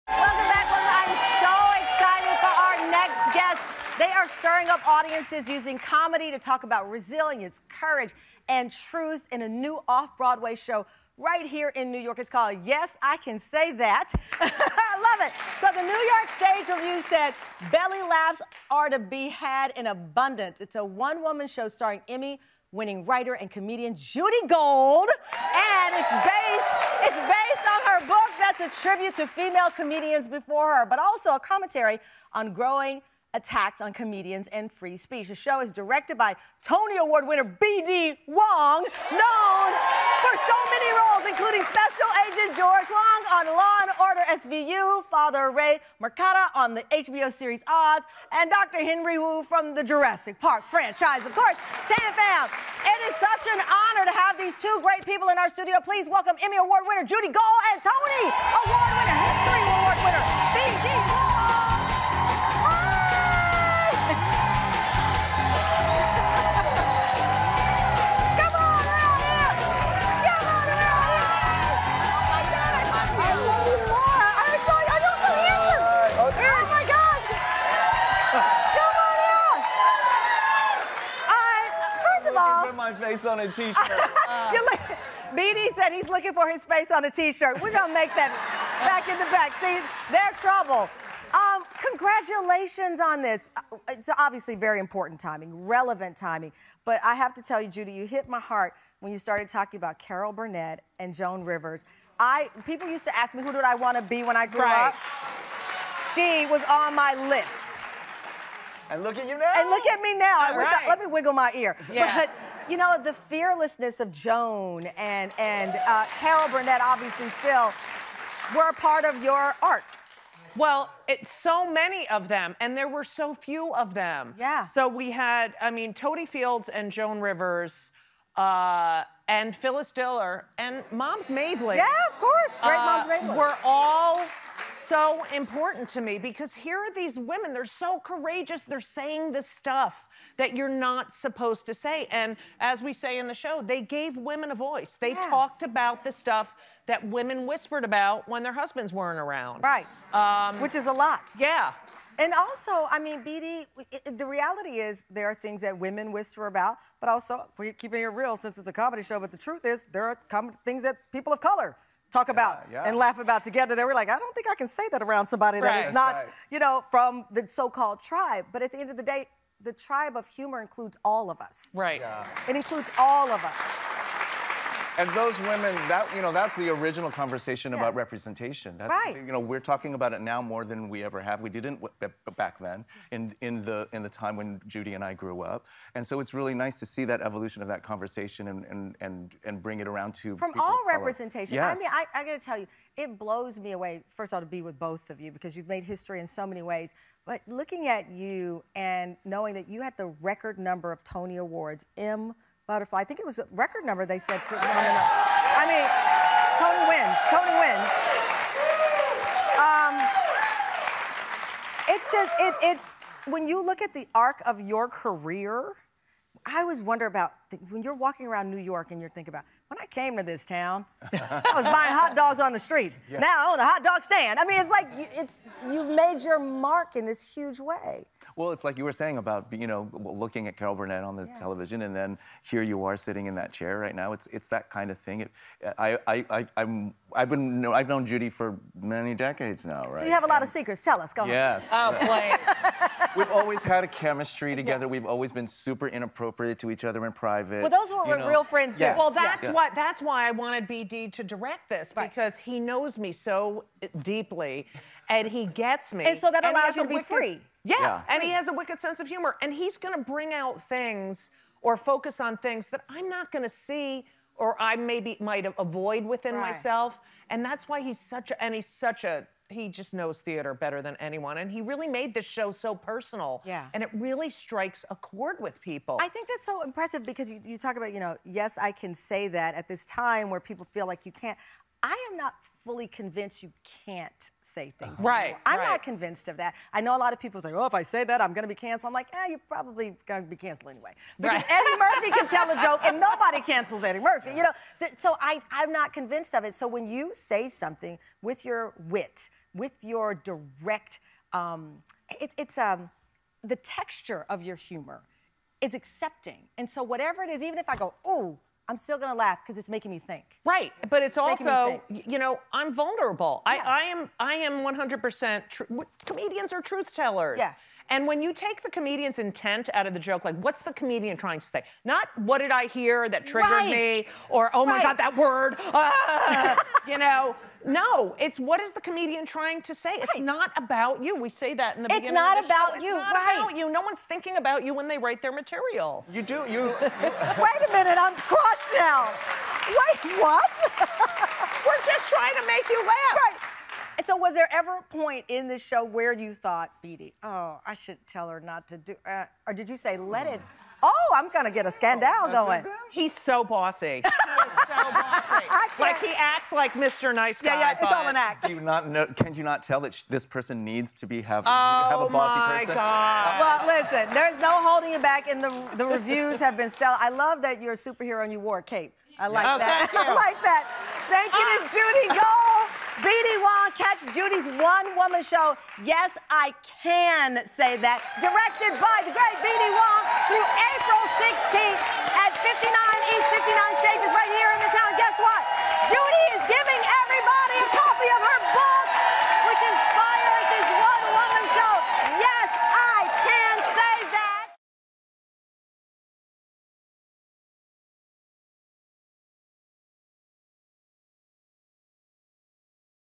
Judy Gold & B.D. Wong On Past Legends Who Gave Women a Voice in Comedy
Comedian Judy Gold and director B.D. Wong join our episode titled “There’s Truth in Jokes” to tell us about their new play, “Yes, I Can Say That!” The play is based on Judy’s book and highlights her quest to return to unfiltered comedic freedom. Judy and B.D. also tell Tamron about the inspirations behind the project and pay homage to legends like Joan Rivers who paved the way for women In comedy.